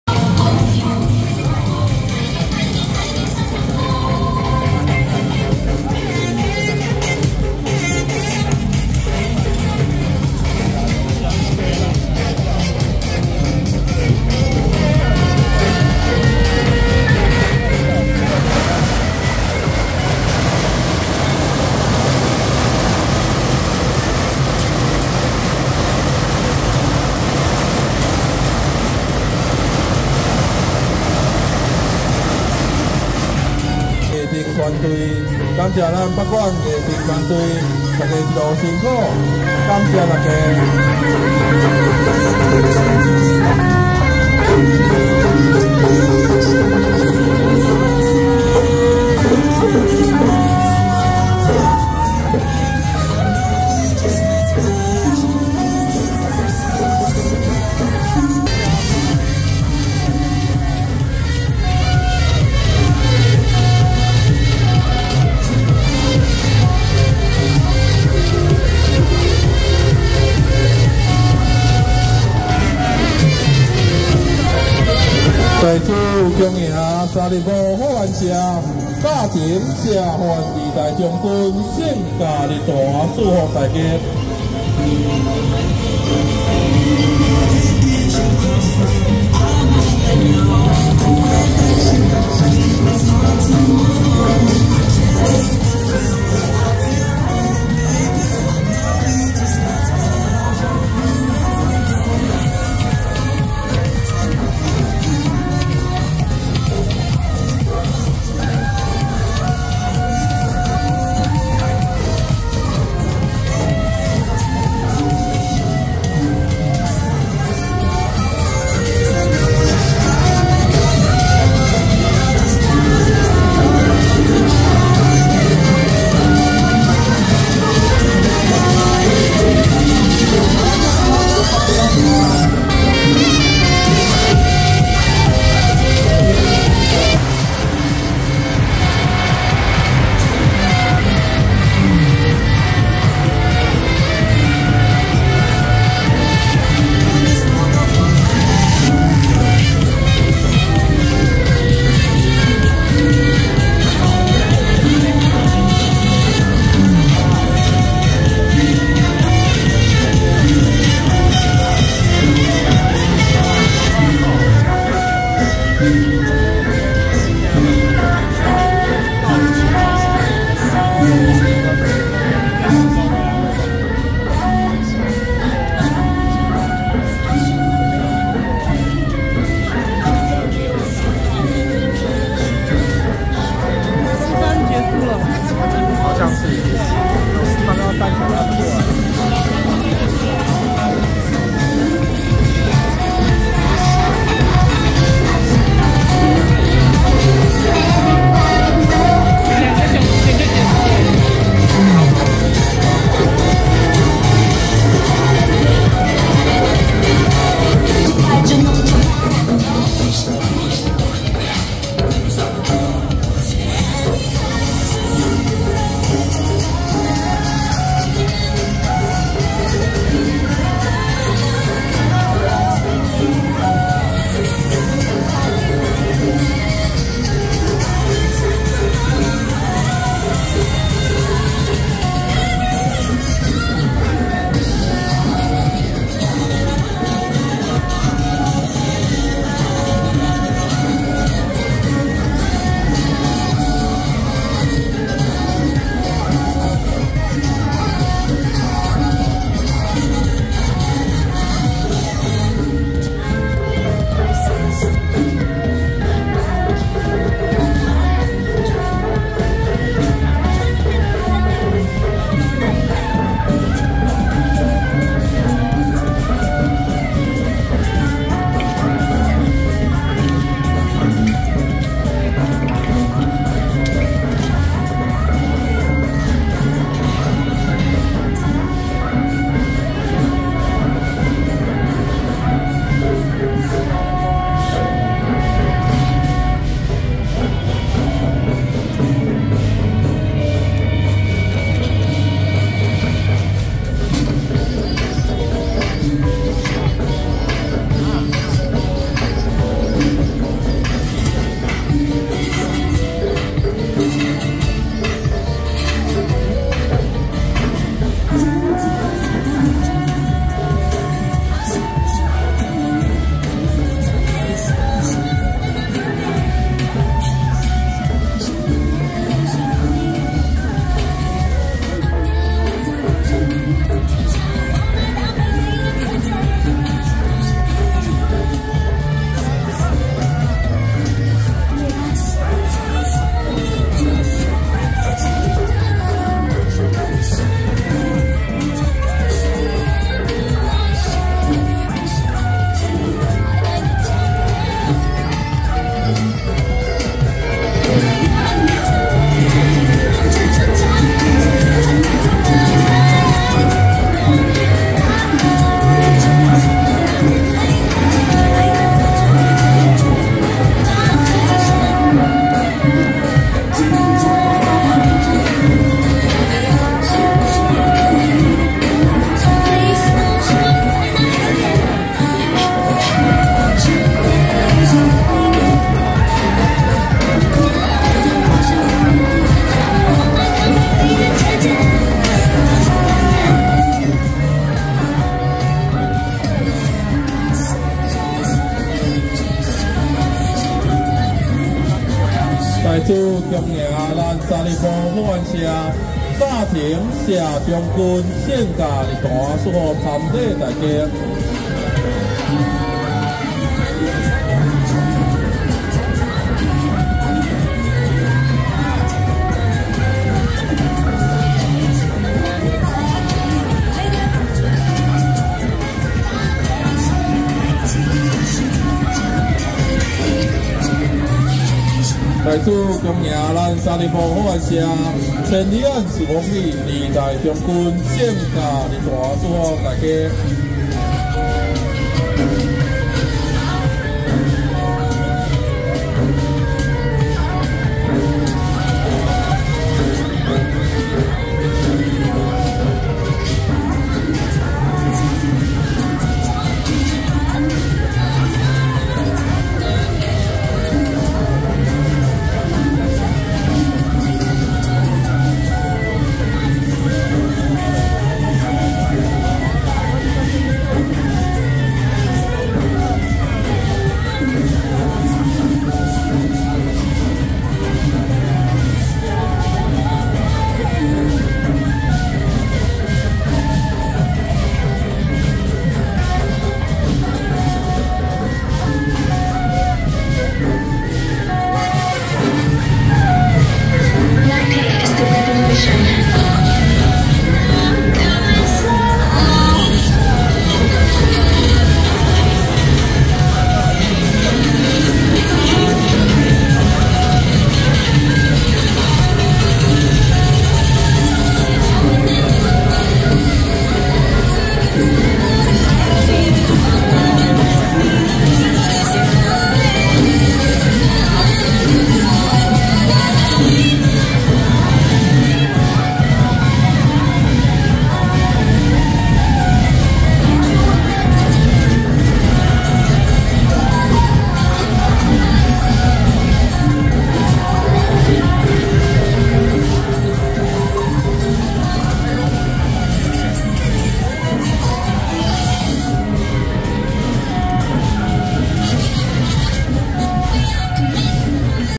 【三重福安社-神將團】~113三重港尾忠義堂金獅團達摩祖師佛誕暨建舘45週年平安遶境
鮮花車外江＆三重福安社-神將團